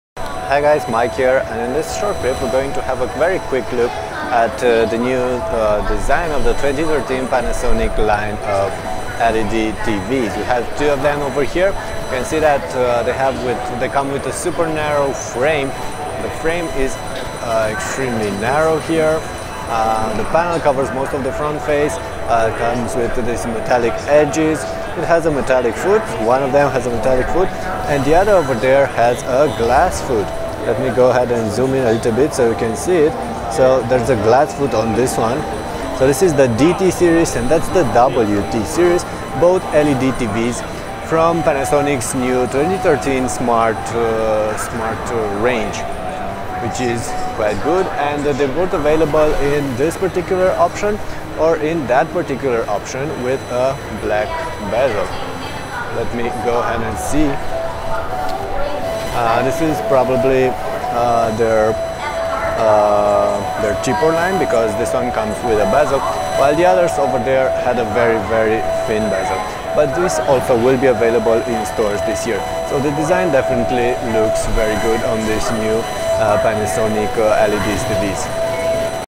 Panasonic 2013 LED TVs at CES
Everybody had 4K TVs at CES, but the mainstream buyers will still choose LED TVs or plasmas, so we're taking a quick look at Panasonic's SmartViera 2013 lineup, DT and WT series, featuring a super narrow metallic bezel design which I like a lot. A glass stand foot completes the stylish design of these TVs.